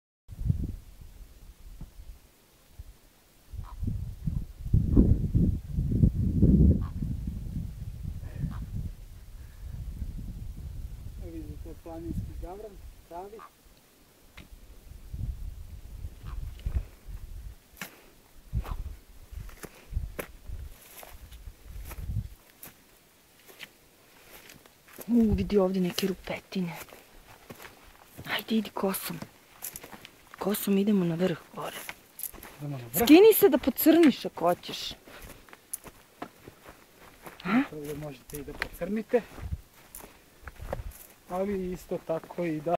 Planina Lisac
Crna Gora, izmedju Pljevalja i kanjona Tare. Video snimila Marijana Cunjak.